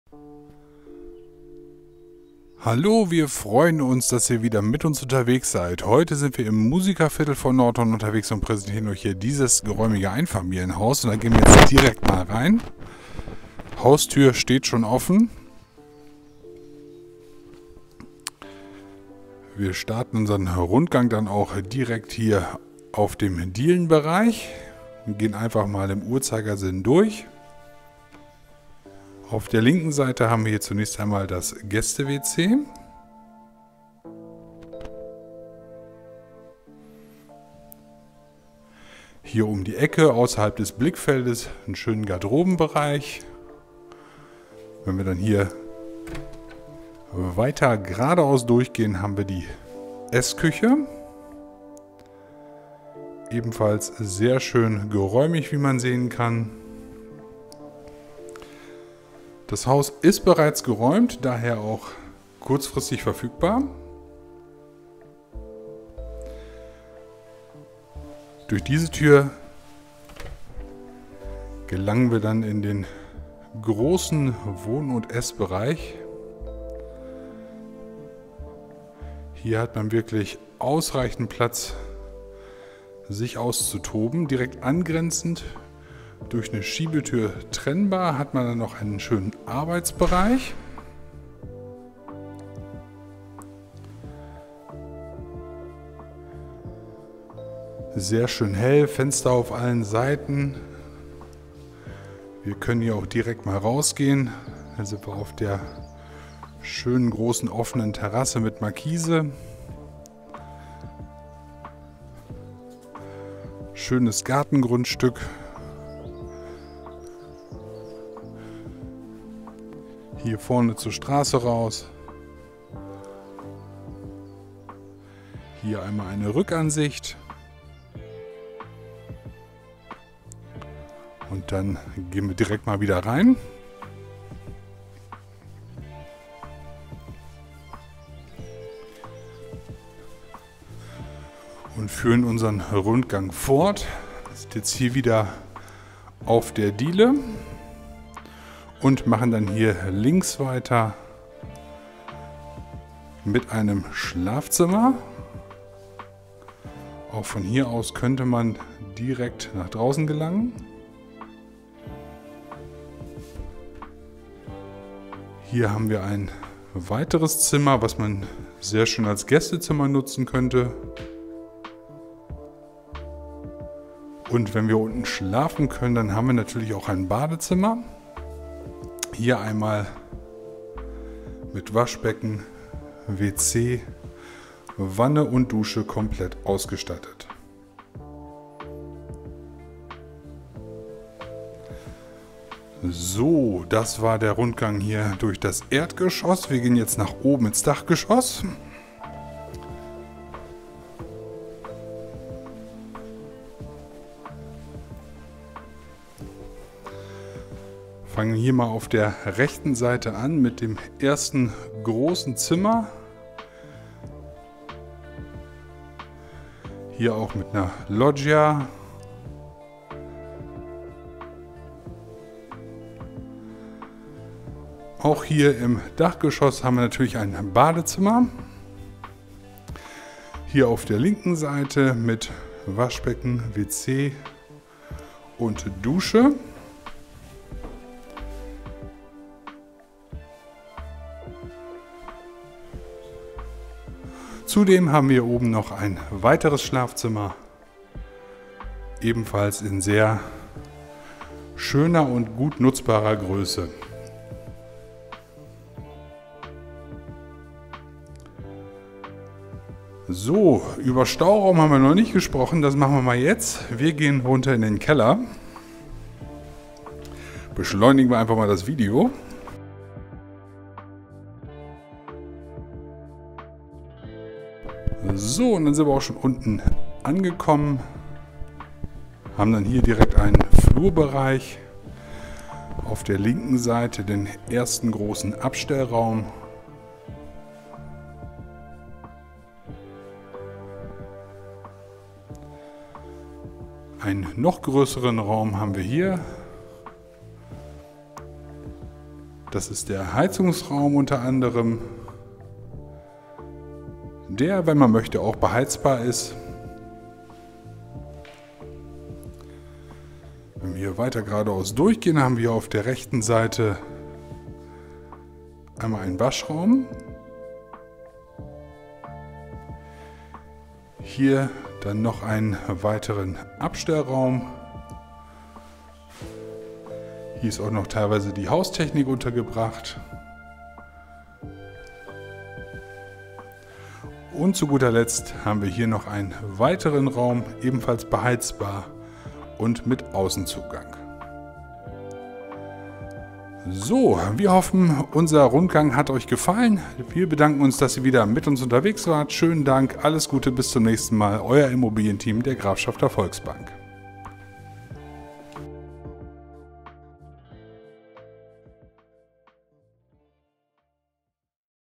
*verkauft* Einfamilienhaus im Musikerviertel von Nordhorn
Schaut euch hier gerne den ersten Rundgang durch die Immobilie an - viel Spaß! Anfragen zu dieser Immobilie bitte per Email unter Angabe eures Namens, eurer Adresse und der Objektnummer an: